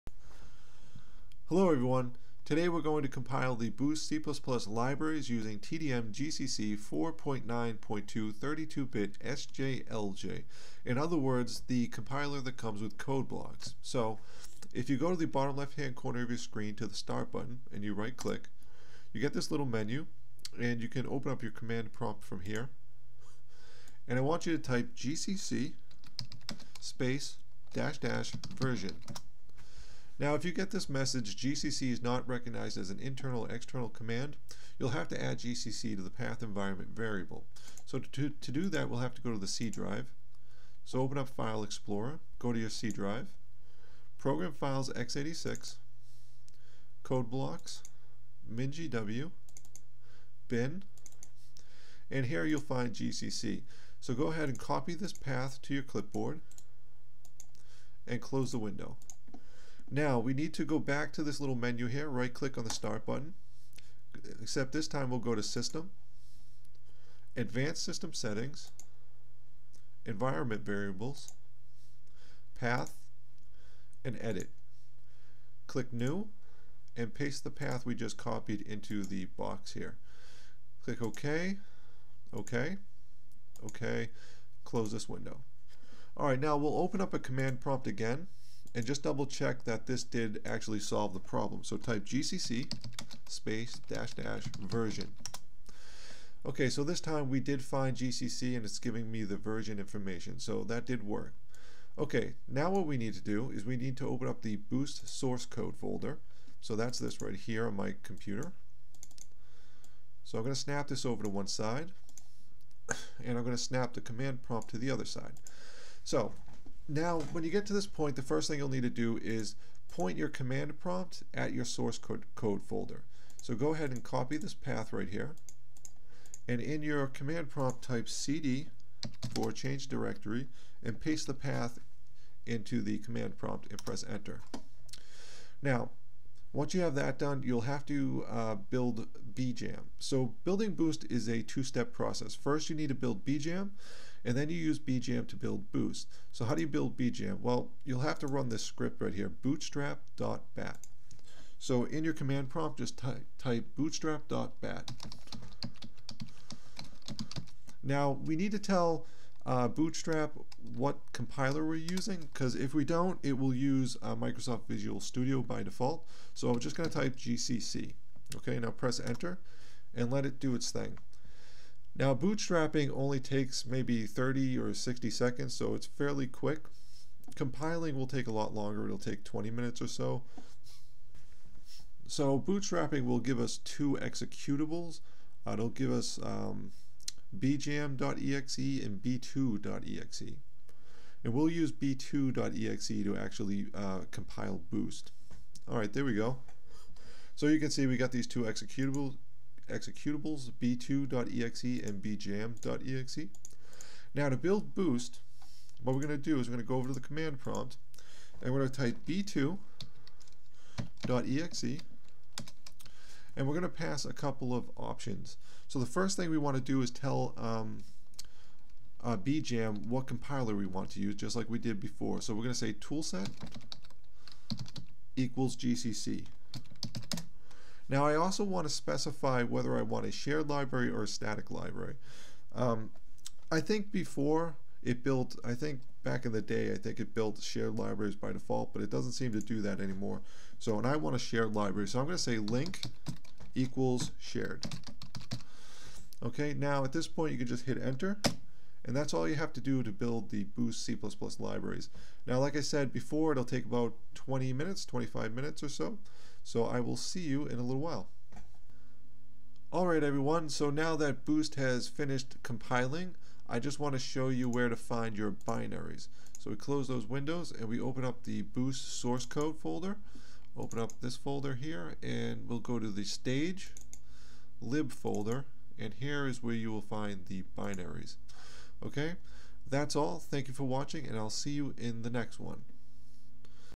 compiling boost with tdm-gcc 4.9 (code blocks)
This tutorial teaches you how to compile the boost c++ libraries with tdm-gcc 4.9 (code blocks).